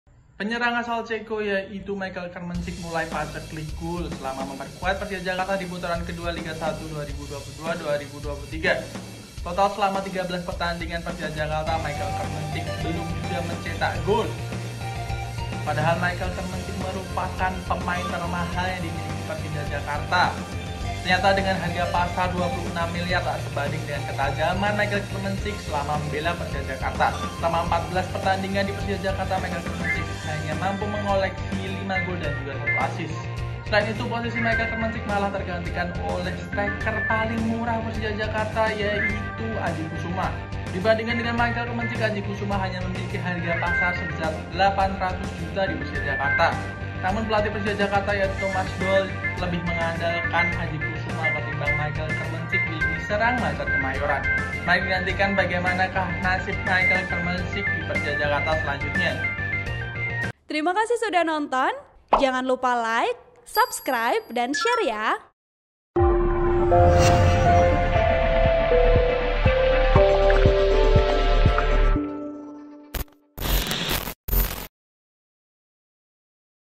Posisi Michael Krmencik Terancam, Thomas Doll Malah Percaya Striker Lokal Termurah Persija Jakarta
TRIBUN-VIDEO.COM - Penyerang asal Ceko, Michael Krmencik tak kunjung mencetak gol untuk Persija Jakarta.

Terakhir kali Michael Kremencik membuat gol saat Persija Jakarta mengalahkan Barito Putera dengan skor 1-0 di pekan ke-9 Liga 1 2022/2023.

Selama 13 pertandingan terakhir Persija Jakarta, Michael Krmencik mengalami paceklik gol.

Ketajaman Michael Krmencik mulai hilang setelah mendapatkan hukuman larangan bermain selama 6 pertandingan Persija Jakarta.

Total Michael Krmencik selama tujuh pertandingan Persija Jakarta tidak mencetak gol.

Dalam 14 pertandingan, Michael Krmencik hanya mengoleksi 5 gol dan 1 assist untuk Persija Jakarta.

Padahal Michael Krmencik merupakan pemain termahal Persija Jakarta yaitu dengan harga pasar Rp 26 miliar.

Harusnya harga Michael Krmencik sebanding dengan pundi-pundi golnya untuk Persija Jakarta.

Michael Krmencik malah kalah dengan penyerang lokal baru Persija Jakarta, yakni Aji Kusuma.

Selain itu, Michael Krmencik kalah dengan striker termurah Persija Jakarta yang memiliki harga pasar Rp 869 juta.

Bahkan, pelatih Thomas Doll pernah tidak memberikan menit bermain pada Michael Krmencik ketika Persija Jakarta menang 4-2 dari PSM Makassar.

Dua gol Persija Jakarta ke gawang PSM Makassar dicetak oleh Aji Kusuma.

Thomas Doll lebih mempercayakan lini serang pada Aji Kusuma ketimbang Michael Krmencik.

Michael Krmencik selalu memulai pertandingan dari bangku cadangan Persija Jakarta.

Artikel ini telah tayang di TribunWow.com dengan judul Posisi Michael Krmencik Terancam, Thomas Doll Malah Percaya Striker Lokal Termurah Persija Jakarta,

Penulis: Khistian Tauqid Ramadhaniswara
Editor: Elfan Fajar Nugroho
VP: Firdausy Shabrina R